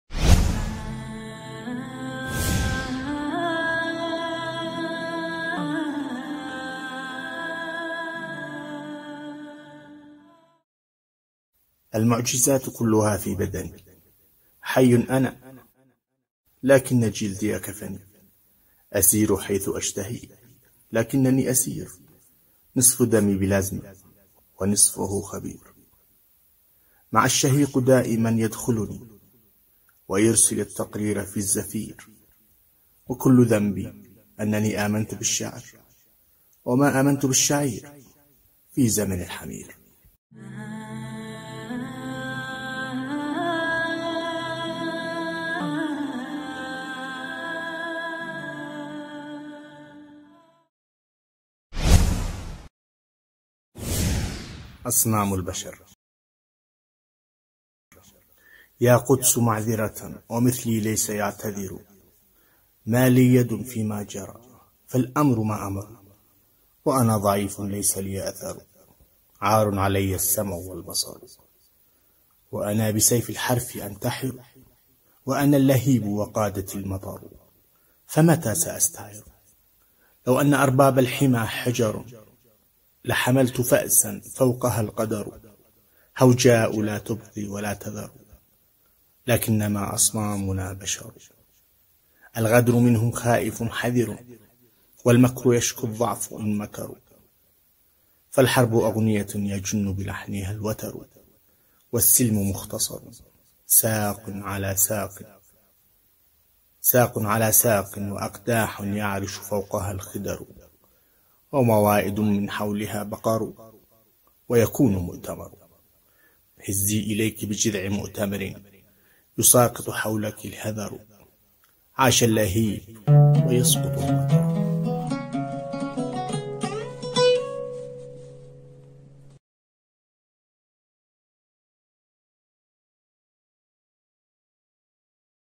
شاهد : زمن 🦓 الحمير 🦓 أحمد مطر يصف زمن التخاذل .، أصنام البشر 🗿
الشاعر الثائر ، أحمد مطر  ، في قصيدته الشهيرة "زمن الحمير" و كذلك قصيدة " أصنام البشر " ..

قصيدة زمن الحمير :

المعجزات كلها في بدني ،

حي أنا لكن جلدي كفني ،

أسير حيث أشتهي لكنني أسير ،

نصف دمي بلازما، ونصفه خبير ،

مع الشهيق دائما يدخلني، ويرسل التقرير في الزفير ،

وكل ذنبي أنني آمنت بالشعر، وما آمنت بالشعير ،

في زمن الحمير..

قصيدة أصنام البشر:

يا قدس معذرة ومثلي ليس يعتذر ،

مالي يد في ما جرى فالأمر ما أمروا ،

وأنا ضعيف ليس لي أثر ،

عار علي السمع والبصر ،

وأنا بسيف الحرف أنتحر ،

وأنا اللهيب وقادتي المطر ،

فمتى سأستعر ؟

لو أن أرباب الحمى حجر ،

لحملت فأسا فوقها القدر ،

هوجاء لا تبقي ولا تذر ؛

لكنما أصنامنا بشر ،

الغدر منهم خائف حذر ،

والمكر يشكو الضعف إن مكروا ؛

فالحرب أغنية يجن بلحنها الوتر ،

والسلم مختصر ،

ساق على ساق ، وأقداح يعر ش فوقها الخدر ،

وموائد من حولها بقر ،

ويكون مؤتمر ؛

هزي إليك بجذع مؤتمر يسا قط حولك الهذر ،

عاش اللهيب ويسقط المطر